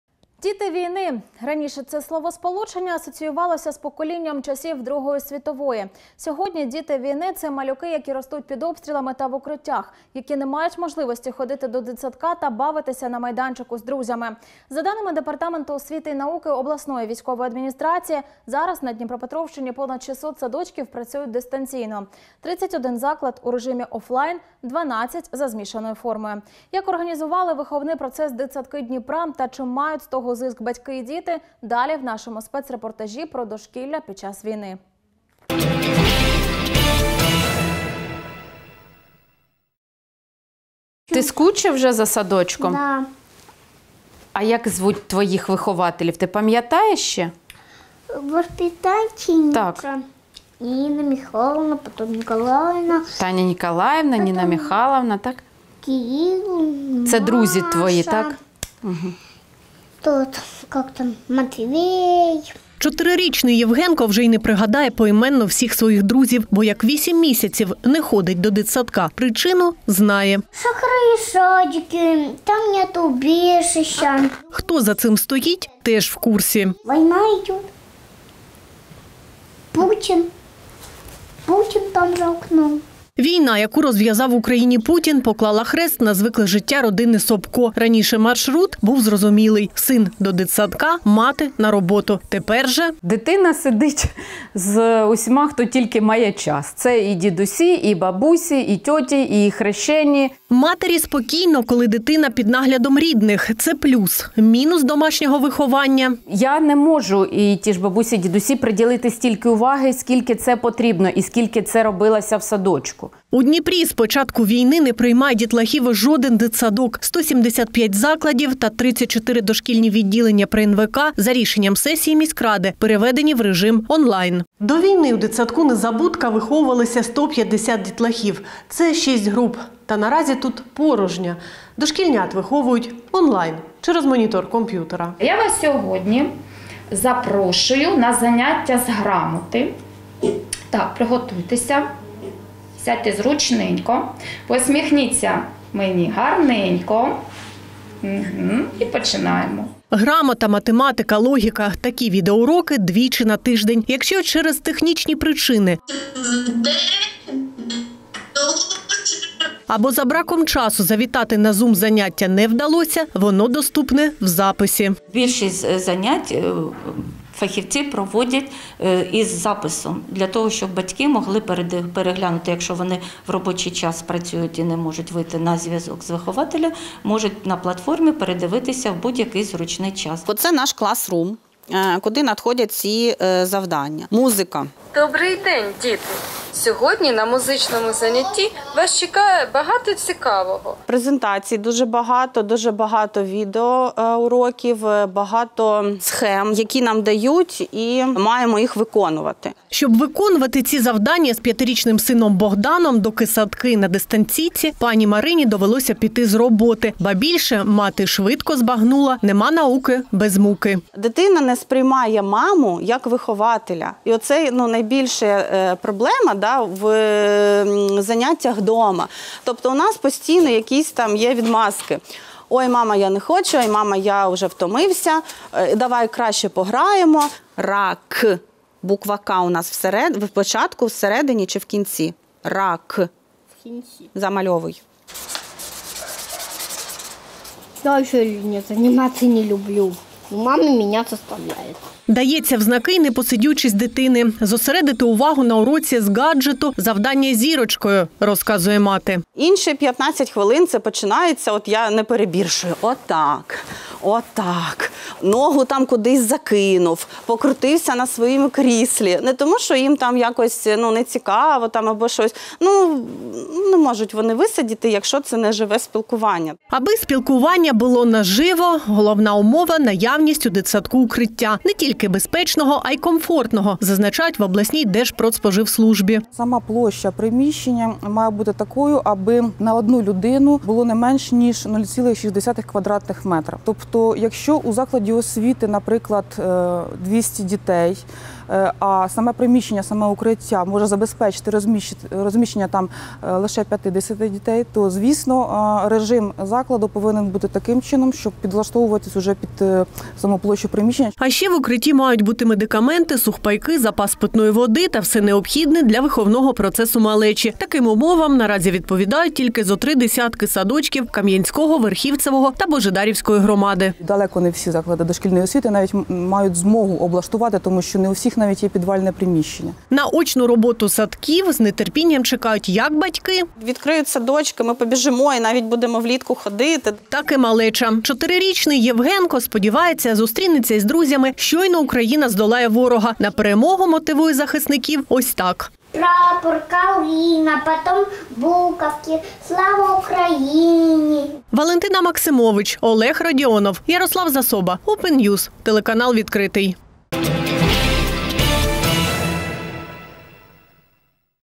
«Дитина не сприймає матір як вихователя»: про дошкілля під час війни та як працюють садки Дніпра
Діти війни. Раніше це словосполучення асоціювалося з поколінням часів Другої світової. Сьогодні діти війни – це малюки, які ростуть під обстрілами та в укриттях, які не мають можливості ходити до дитсадка та бавитися на майданчику з друзями. За даними департаменту освіти й науки обласної військової адміністрації, зараз на Дніпропетровщині понад 600 садочків працюють дистанційно, 31 заклад – у режимі офлайн, 12 – за змішаною формою. Як організували виховний процес дитсадки Дніпра та чи мають з того зиск батьки й діти – далі в нашому спецрепортажі про дошкілля під час війни. _______________________________________________